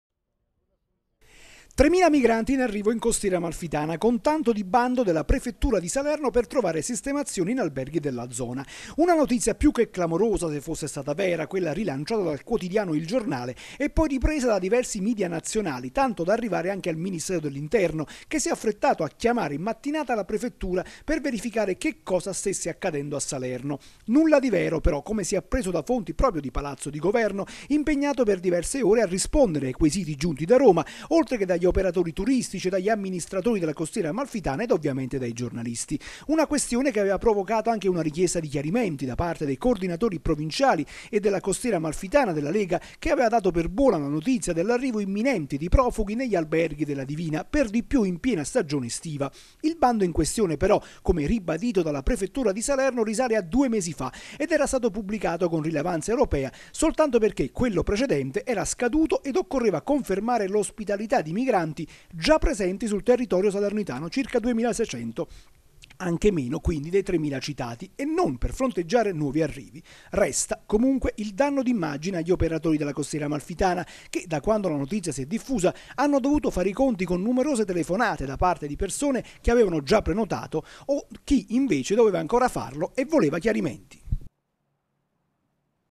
Migranti in arrivo in Costiera Amalfitana, ma non era vero
Una notizia vecchia di due mesi e, per di più, priva di ogni fondamento. L'allarme lanciato questa mattina dal quotidiano Il Giornale e ripreso da diversi media nazionale sull'arrivo di 3000 migranti in Costiera Amalfitana è completamente falso. La precisazione viene dalla Prefettura di Salerno. Intanto, però, il danno d'immagine per il turismo è stato fatto. (050618)